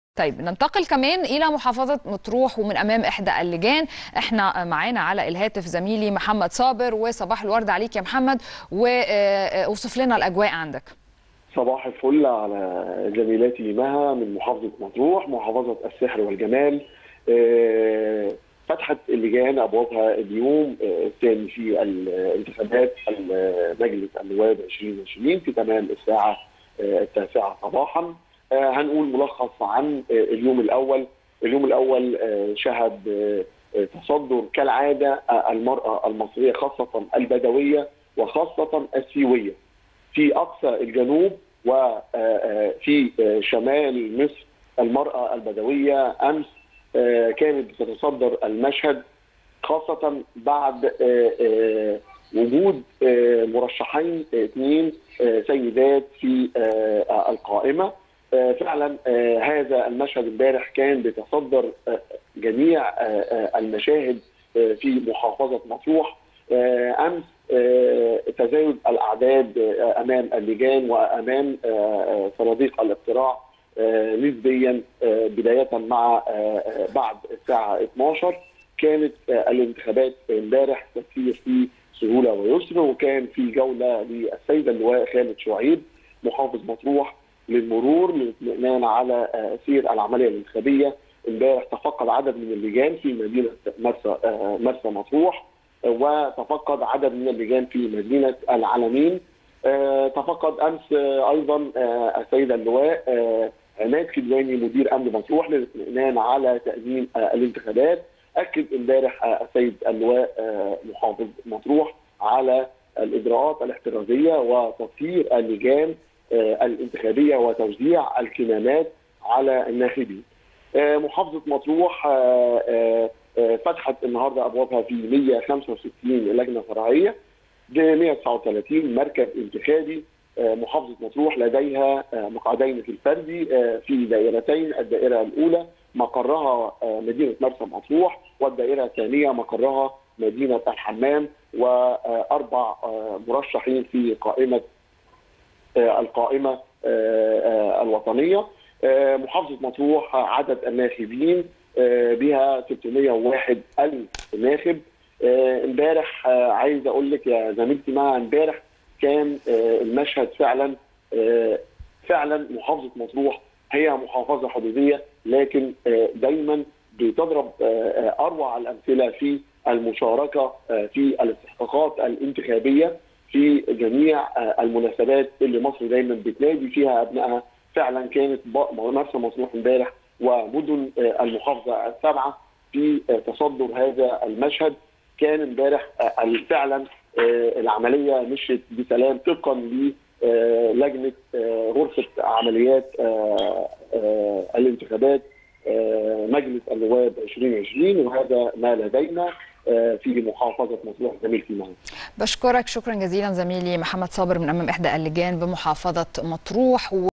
من محافظة مطروح.. مراسل TeN ينقل لكم أجواء الدقائق الأولى لليوم الثاني من انتخابات مجلس النواب
مجلس_النواب- الانتخابات_البرلمانية - من محافظة مطروح.. محمد صابر مراسل TeN ينقل لكم أجواء الدقائق الأولى لليوم الثاني من انتخابات مجلس النواب حيث فتحت أبواب اللجان لليوم الثاني أمام الناخبين للتصويت في انتخابات مجلس النواب بمرحلتها الأولى